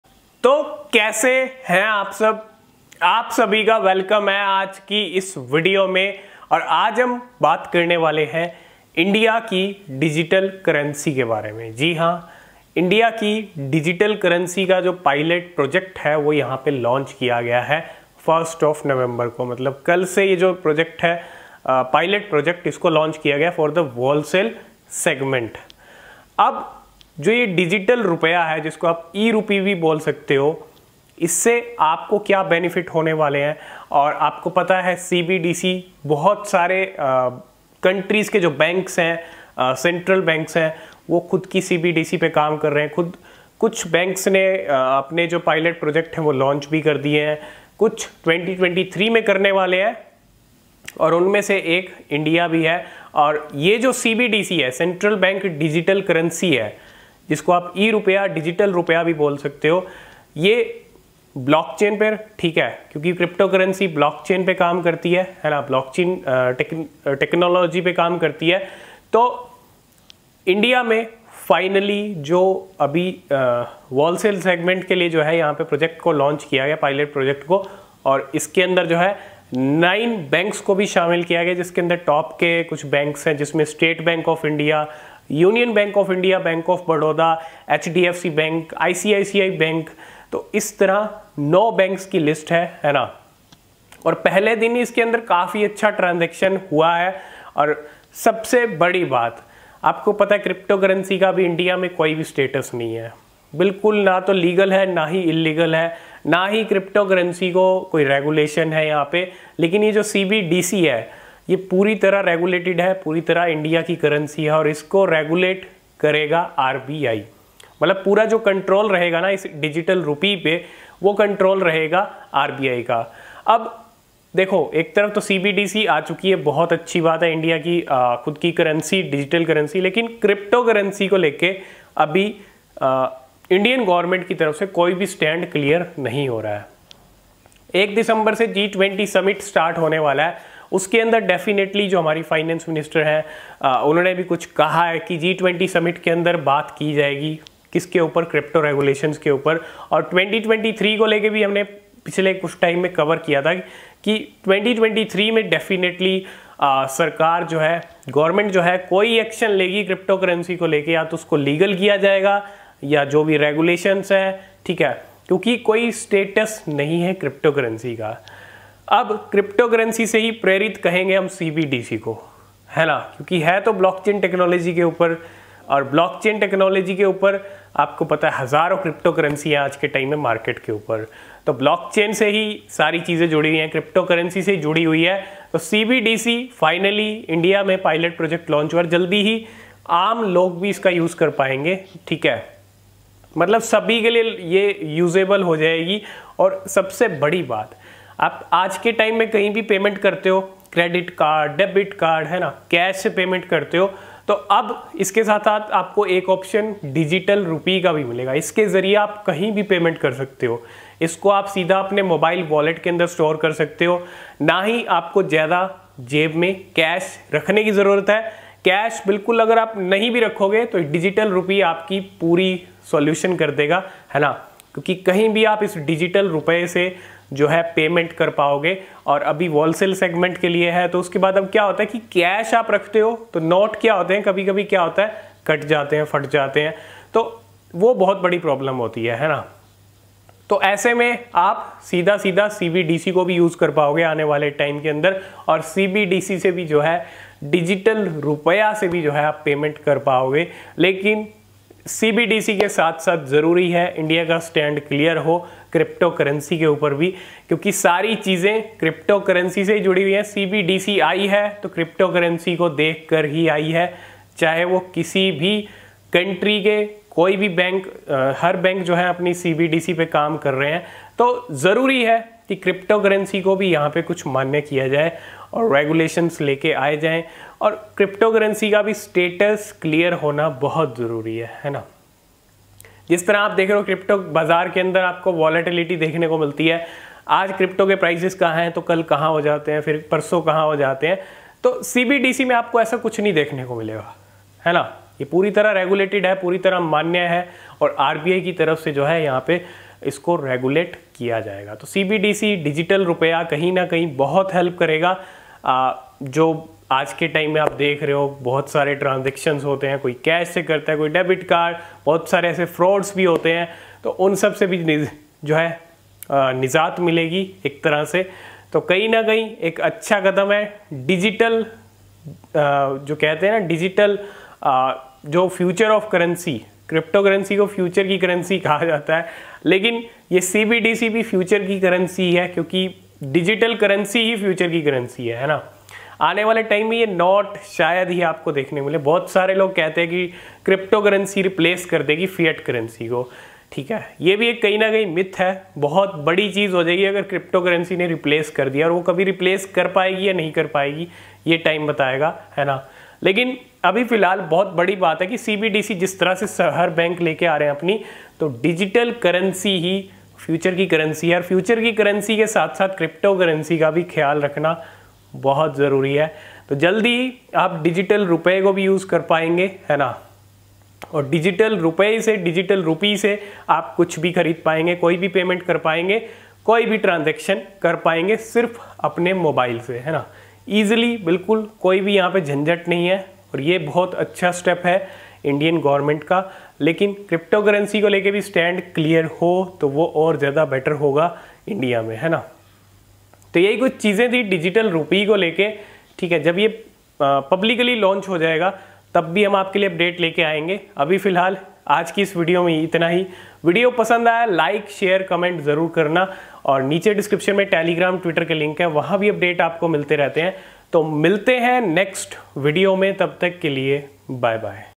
India की Digital Currency आ गई | पर Cryptocurrency का कोई Status नहीं
Hey Everyone

My Equipments

2nd Lens